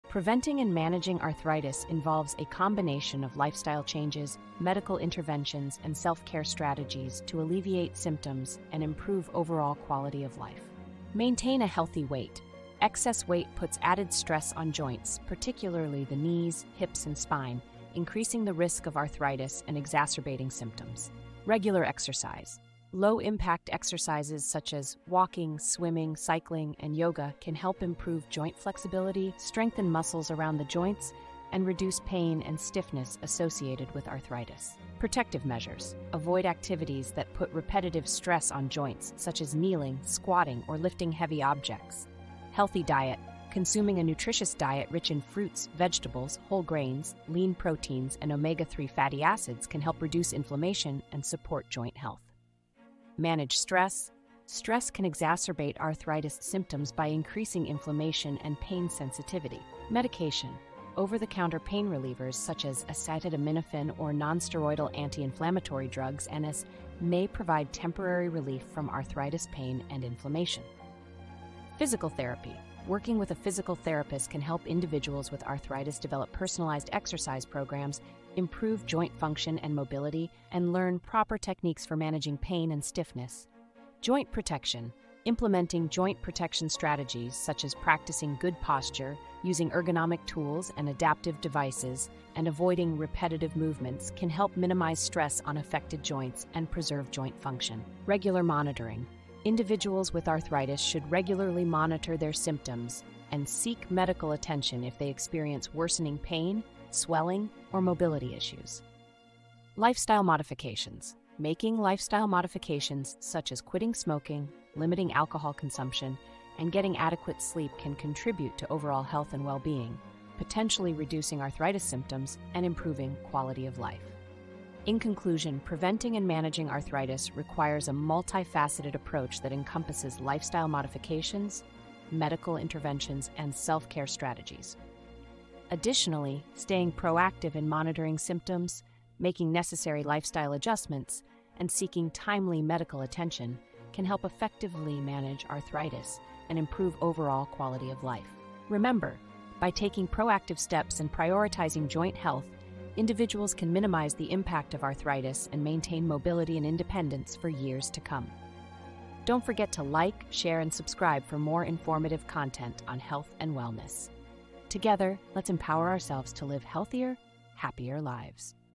Arthritis Management - A Comprehensive Guide
Preventing and managing arthritis requires a comprehensive approach that includes lifestyle changes, medical interventions, and self-care strategies. Maintaining a healthy weight, engaging in regular exercise, and eating a balanced diet can help prevent arthritis and alleviate symptoms. Protecting joints from repetitive stress, managing stress levels, and seeking timely medical advice are essential aspects of arthritis management. Utilizing assistive devices, such as ergonomic tools or joint braces, can also reduce strain on affected joints and improve mobility. By adopting these strategies, individuals can effectively prevent and manage arthritis, promoting better joint health and overall well-being.